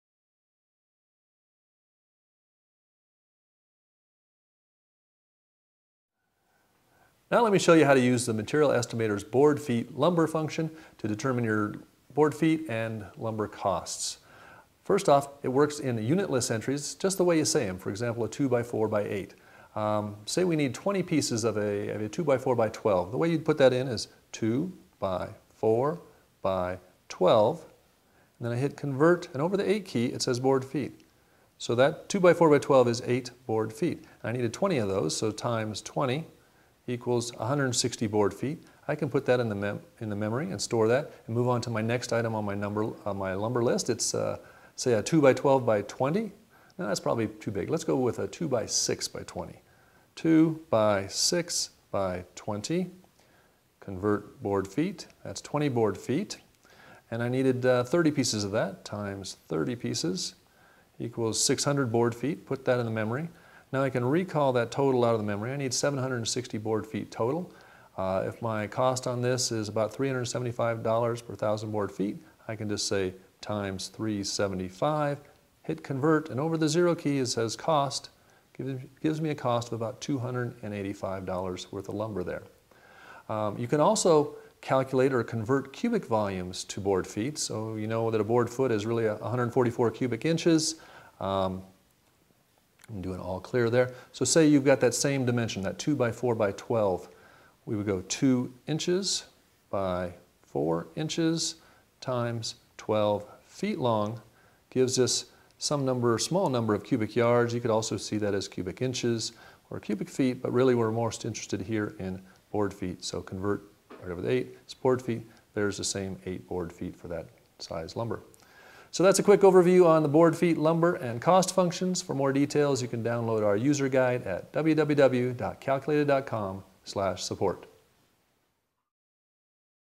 How to Calculate Board Feet Lumber and Cost | Material Estimator
See how the Material Estimator calculator's built-in board foot lumber calculation lets you tally up all your dimensional lumber to get total board feet and costs. This quick overview briefly shows how to use this construction-math power tool to do all your construction jobsite material calculations far more quickly, simply and accurately to improve your productivity and profitability.